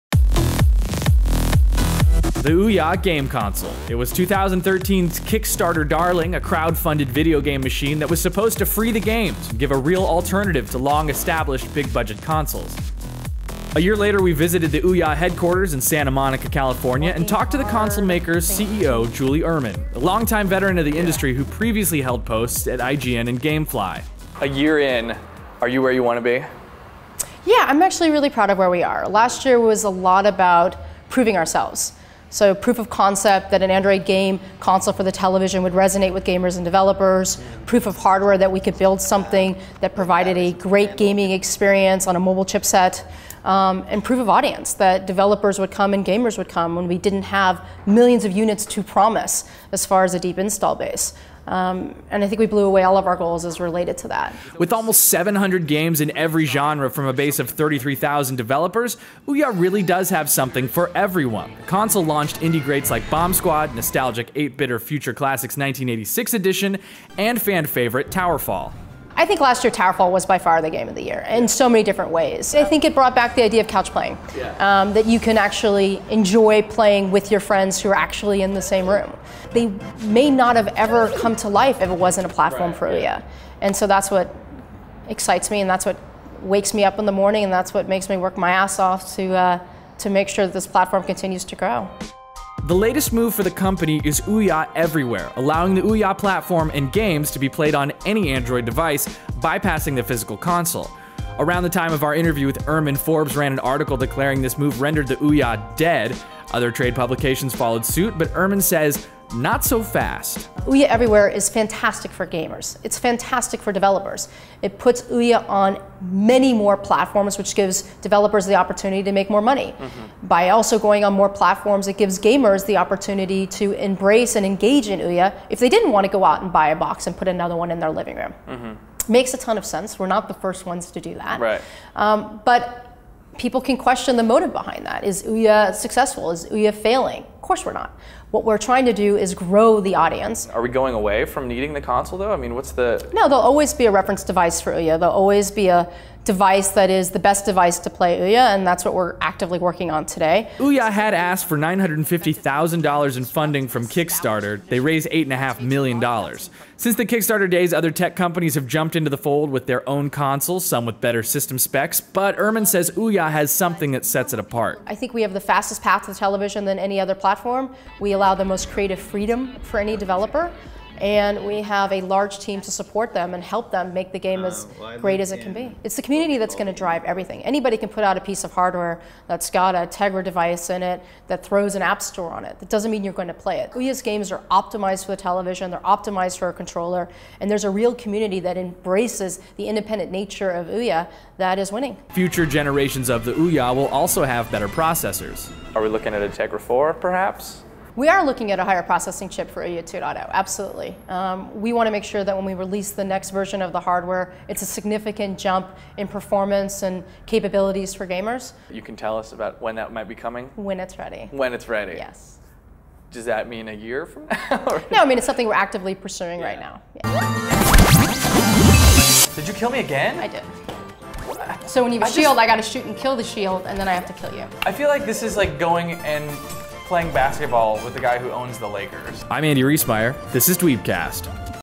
Interview: OUYA CEO Julie Uhrman | DweebCast | OraTV
OUYA CEO Julie Uhrman sits down to talk about where the company is a year after launch. Despite negative press reports that the streamlined console may no longer be viable, Uhrman insists that not only is the console still a great device, but its community of gamers and developers is rapidly growing.

Hosted by @AndyRiesmeyer, Dweebcast is a show for Dweebs, by Dweebs. We're goin' deep into the world of comics, sci-fi, movies, games & more!